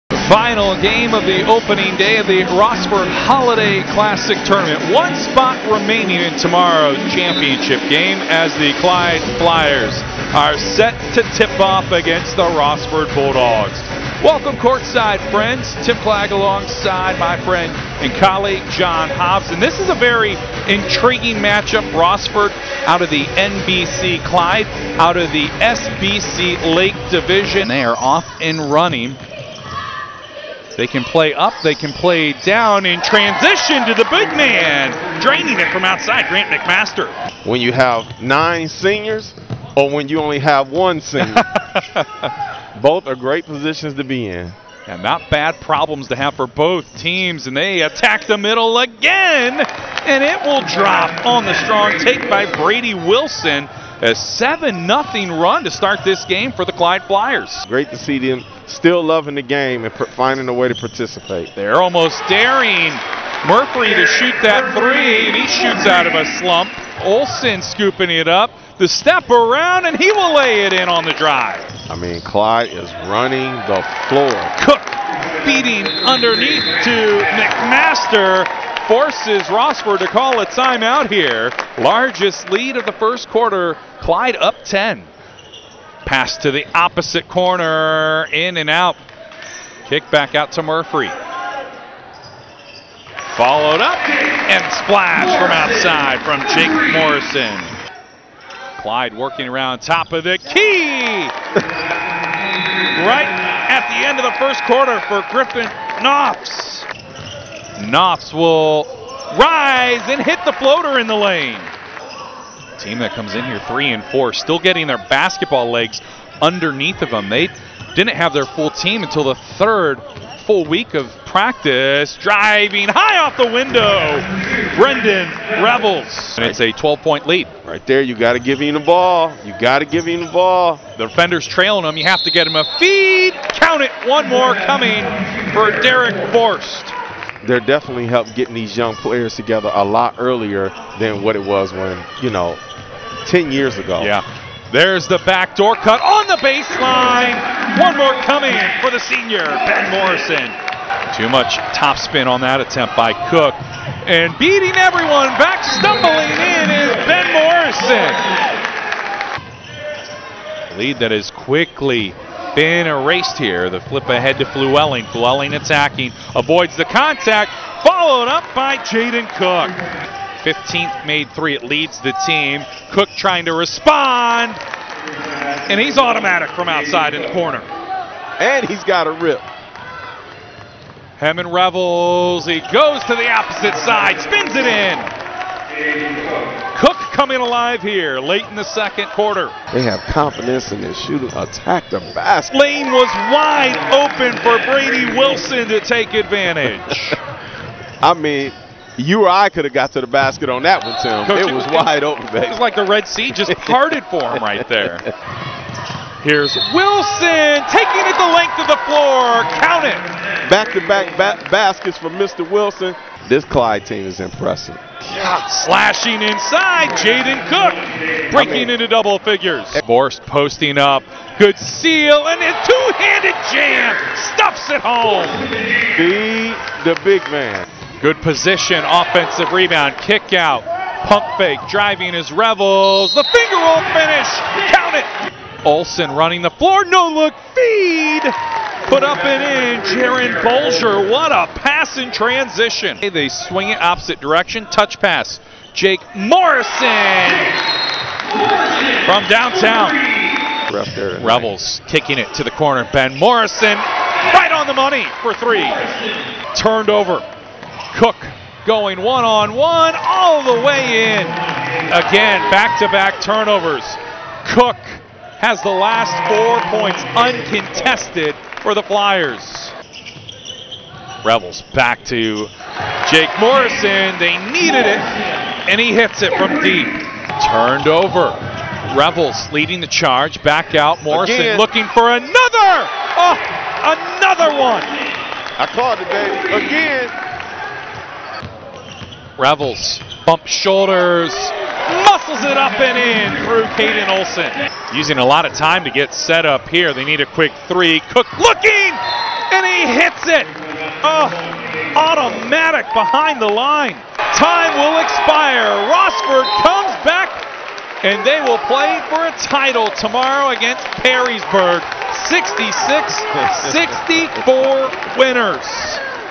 Broadcaster Tim Clagg's highlights Clyde vs Rossford men's bball 2021-2022 highlights
Tim Clagg's broadcast play-by-play highlights from from the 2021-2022 basketball season